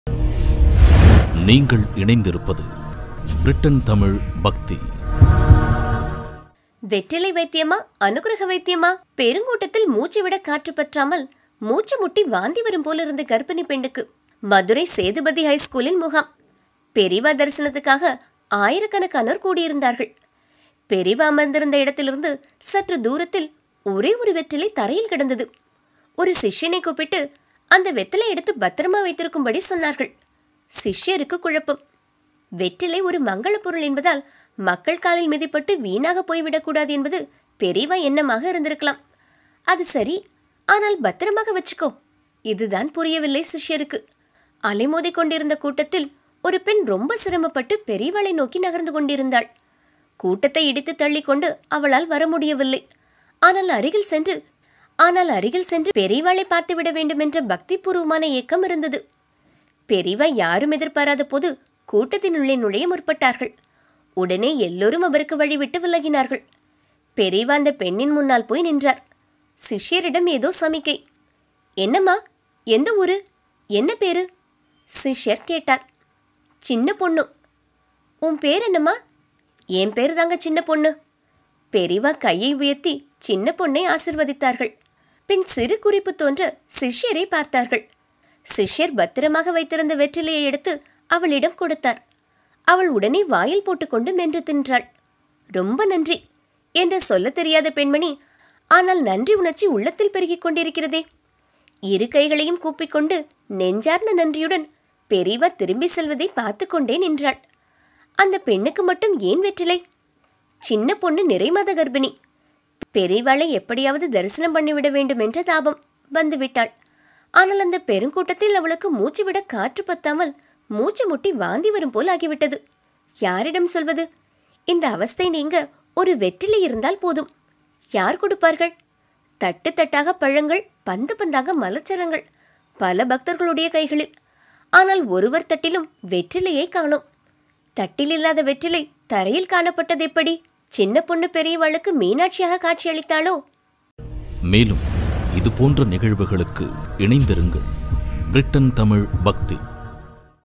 வெற்றிலை வைத்யமா? அனுகிரஹ வைத்யமா ?
வெற்றிலைக்கு தலைசுத்தலை நிறுத்தக்கூடிய சக்தி உண்டான்னு யாருக்கும் தெரியலே.
அந்த வெத்தலைகளை எடுக்கலை.அதே சமயம் அது மங்களகரமான பொருள் என்பதால் அதை மிதிக்காம கொஞ்சம் நகர்ந்து வந்துண்டு இருந்தா.
அந்த அலம்பி வைச்ச வெத்தலைகளை எடுத்து அவாகிட்டே குடு. அதை அந்த கர்ப்பஸ்த்ரீக்கு தரச்சொல்லு!' உத்தரவிட்டார் மகாபெரியவா.
வெற்றிலைகளை வாங்கி மெதுவா மென்னு தின்ன ஆரம்பிச்ச அந்தக் கர்ப்பிணி பெண்ணுக்கு தலைசுத்தல் சட்டுன்னு நின்னுது.